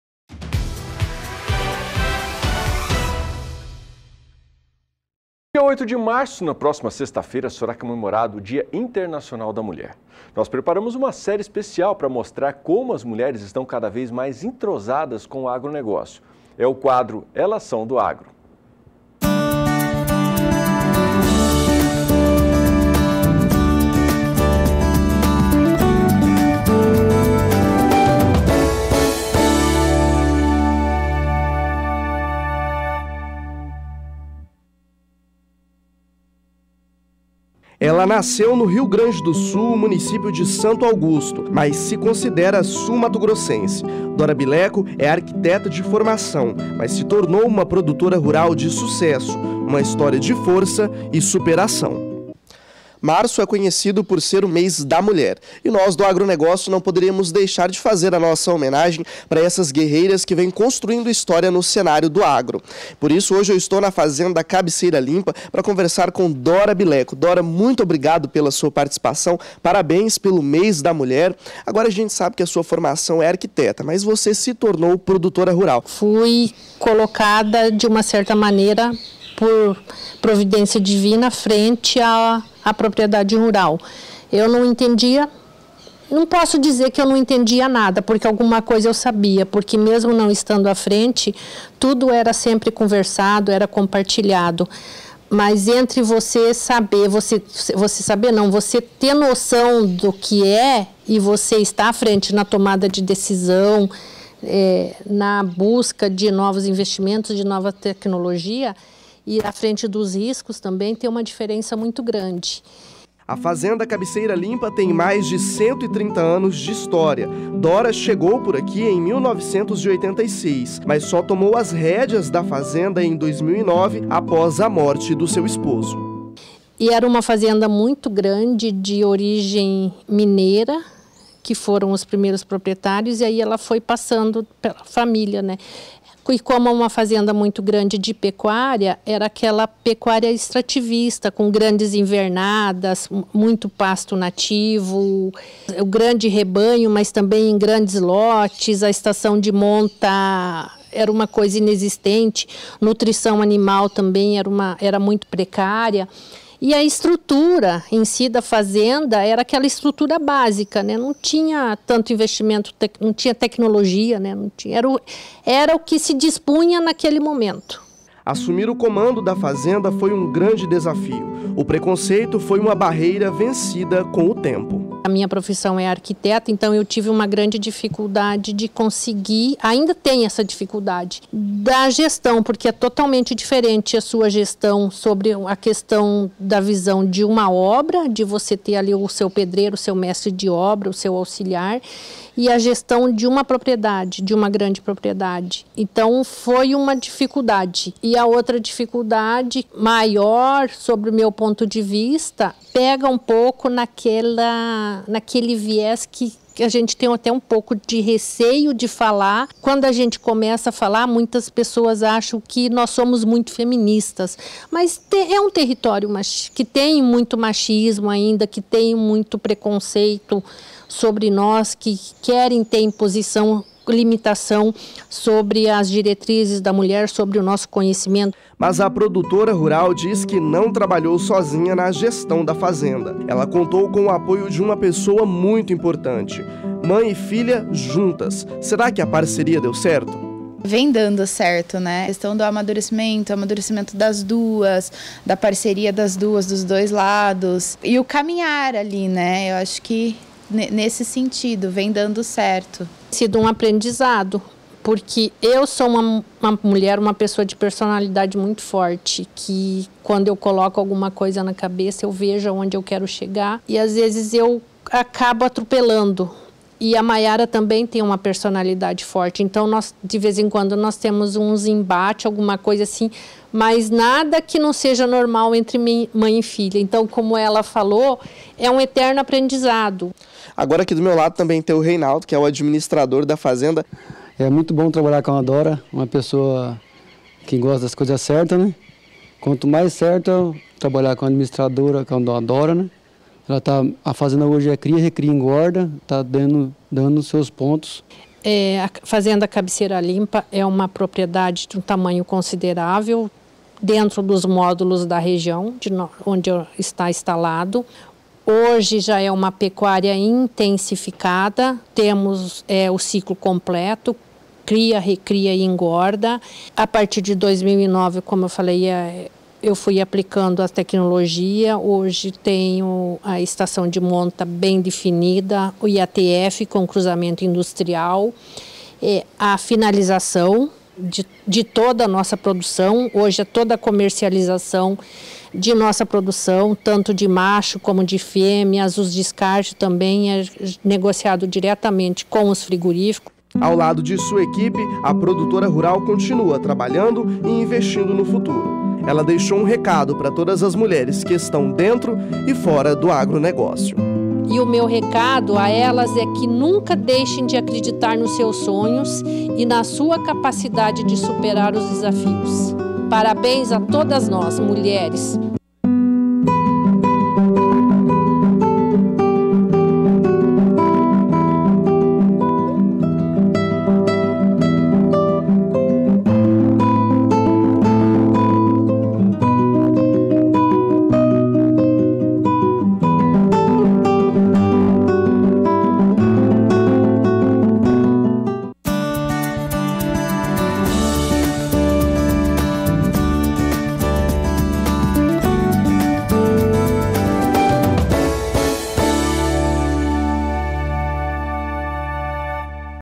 Dora Bileco é exemplo de mulher do agronegócio em MS
No dia 8 de março, sexta-feira, será comemorado o Dia Internacional da Mulher, e em comemoração a essa data, o Canal do Boi fez uma série especial de reportagens, é o quadro “Elas são do Agro”. O repórter Vinícius Souza, conversou com a produtora rural Dora Bileco, sobre a atuação da mulher no agronegócio. Acompanhe.